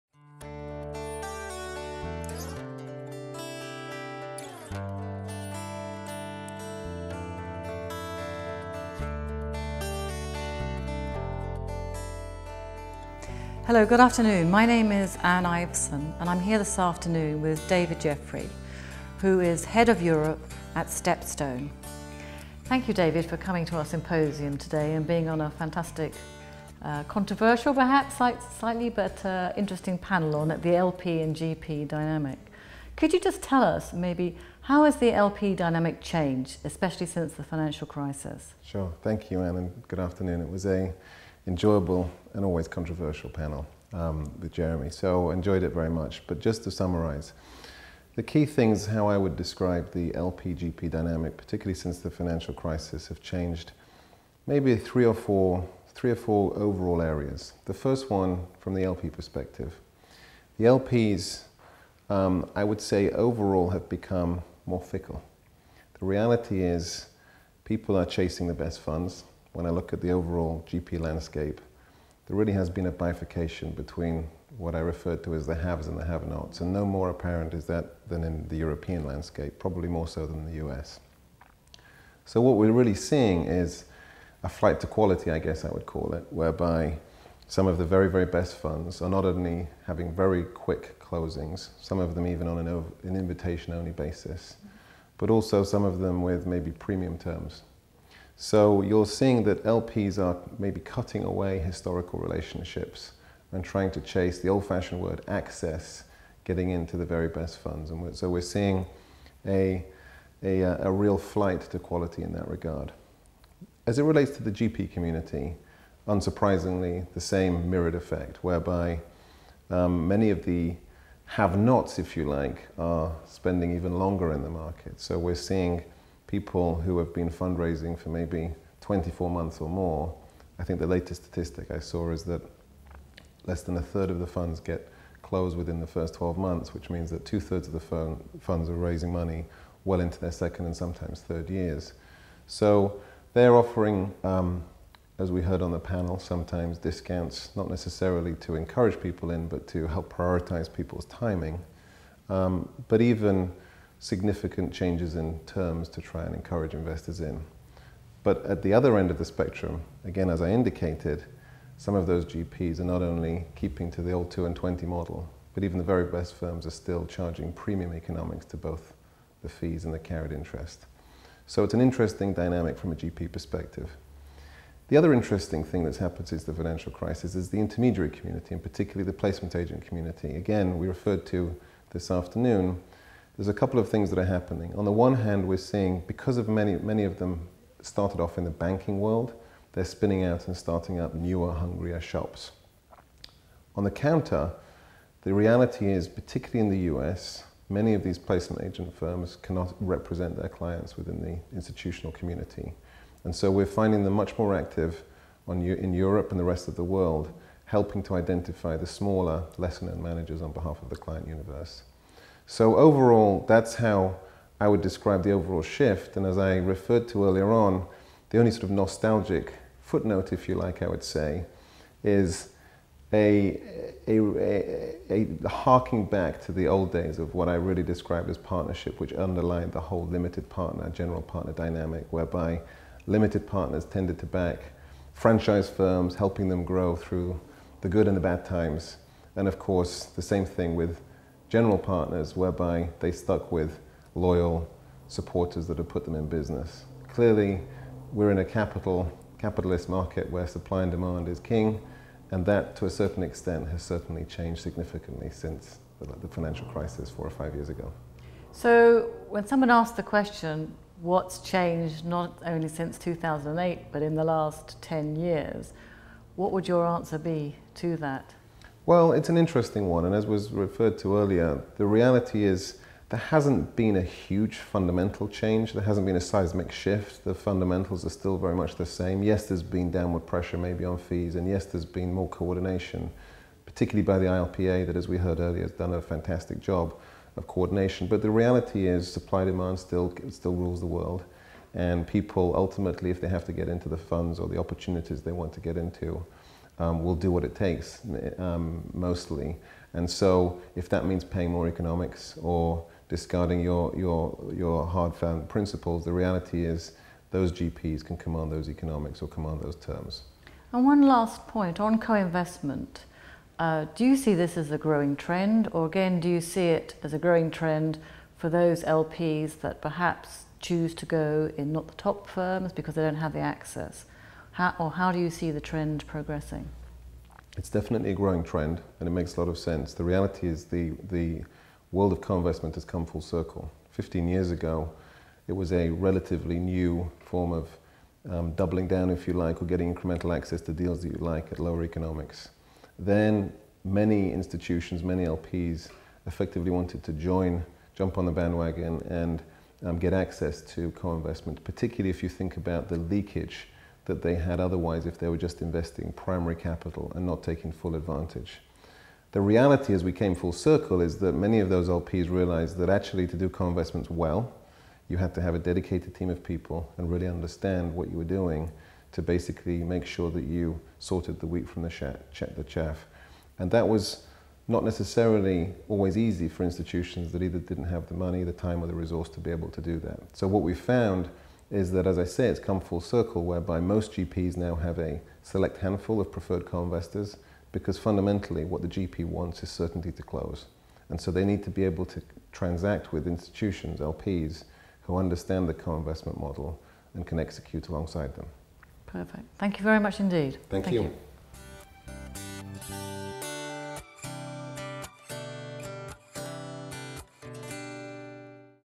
Interview with David Jeffrey, Partner StepSone
David Jeffrey, Partner StepSone, on the continuously evolving LP-GP relationship and what has changed most since the crisis.